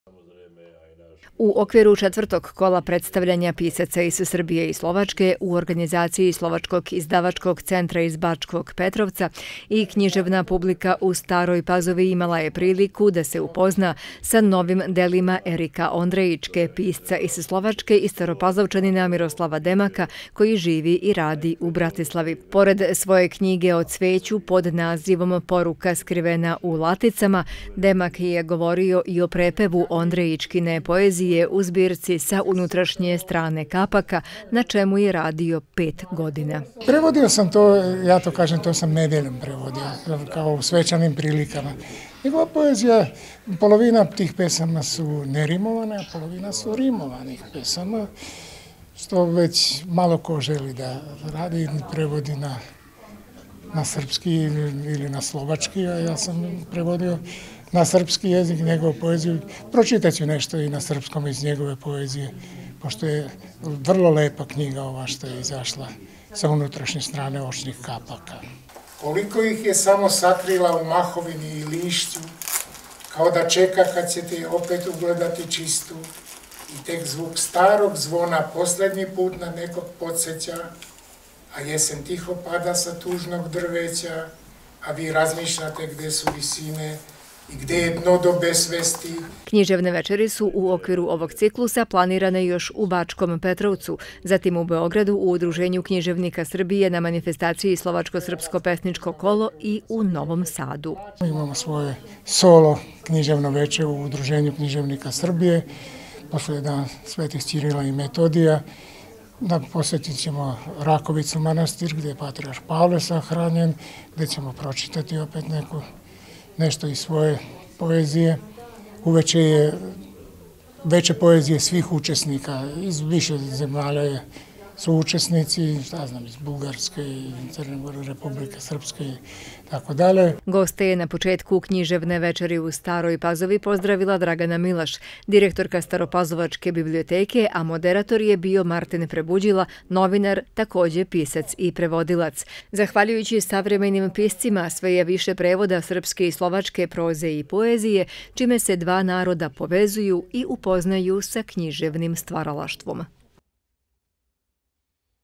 Novi prevodi slovačke poezije na srpski jezik
U poslednje vreme zahvaljujući savremenim piscima i prevodiocima, sve je više prevoda srpske i slovačke poezije čime se dva naroda povezuju i upoznaju sa književnostima. Narodna biblioteka Dositej Obradović iz Stare Pazove sinoć je bila domaćin književne večeri na kojoj su predstavljeni najnoviji naslovi Miroslava Demaka i Erika Ondrejičke.  Već u četvrtak, ali u parohijskom domu Slovačke evangeličke crkve na redu je novo književno veče kada će biti predstavljena Demakova knjiga o cveću iz Slovačke pod nazivom Poruka skrivena u laticama.